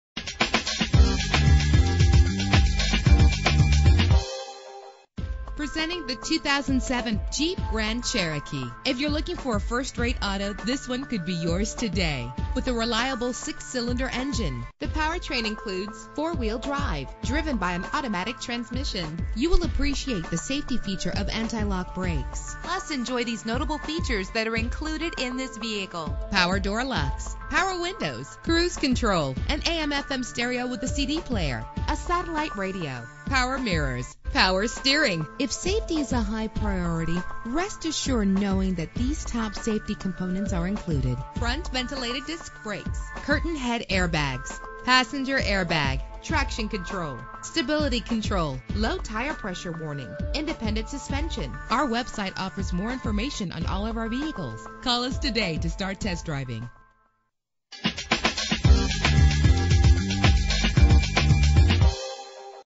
2007 Jeep Grand Cherokee - Lewisville TX
Year: 2007
Make: Jeep
Model: Grand Cherokee
Trim: Laredo
Engine: 3.7 liter 6 cylinder 12 valve
Transmission: Automatic
Color: Green
Mileage: 88828
Address:
1024 South I-35 East, Exit 450